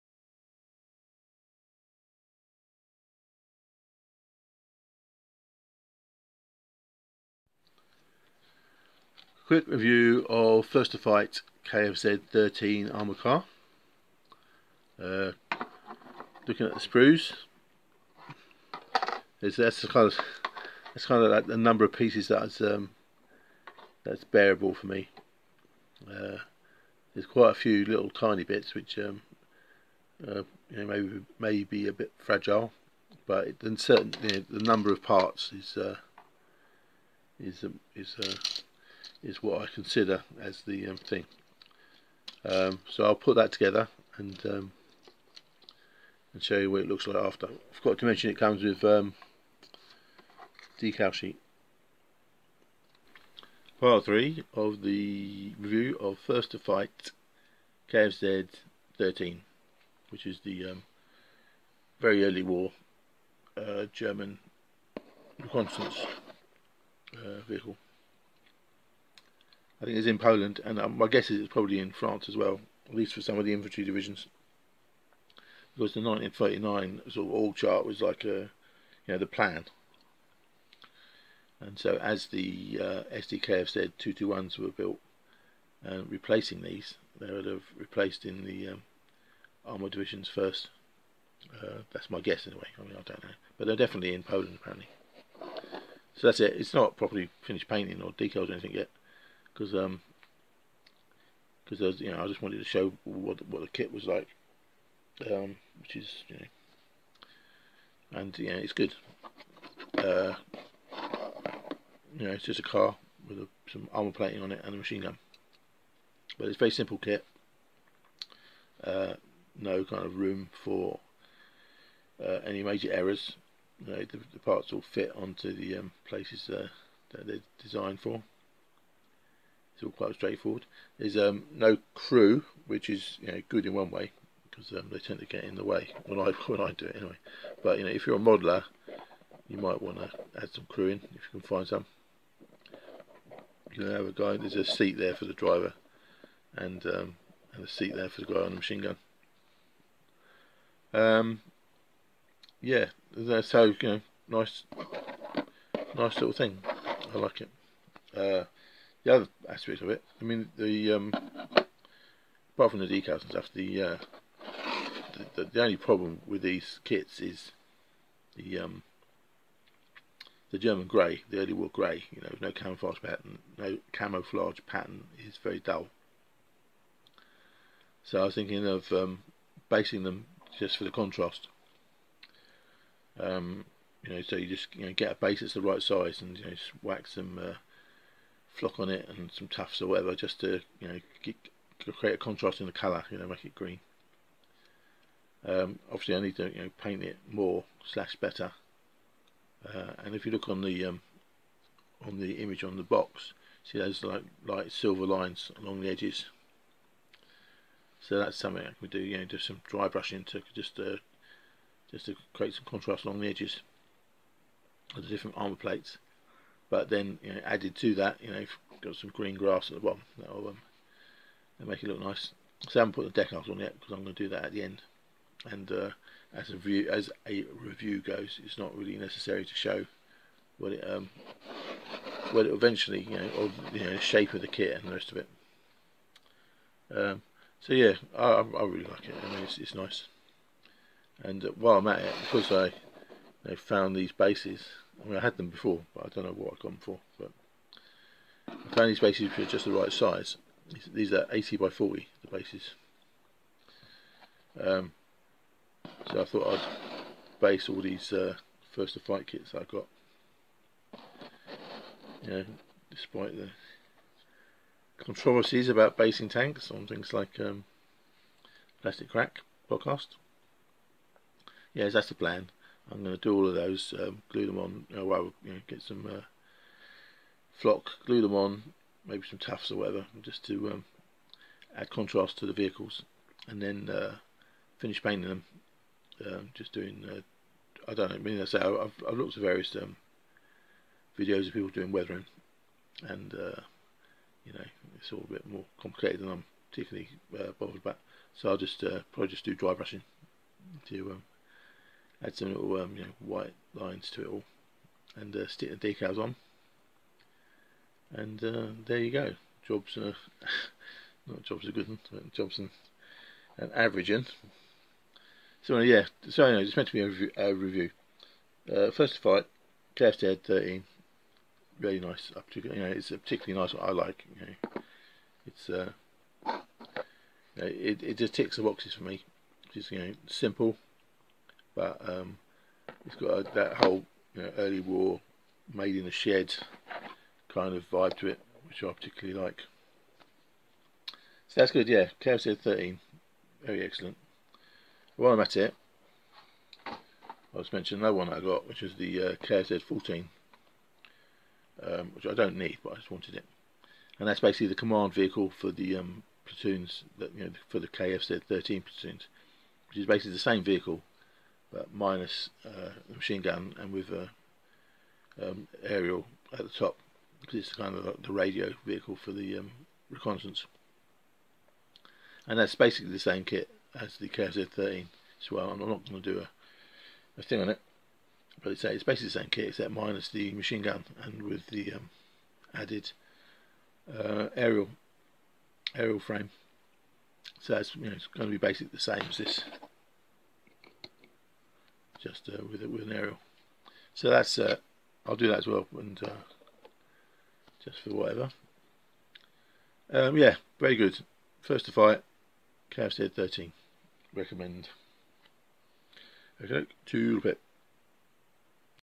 Review: 1:72 First To Fight Kfz.13
A review of 1:72 'First To Fight' Kfz. 13 (and sort of Kfz.14)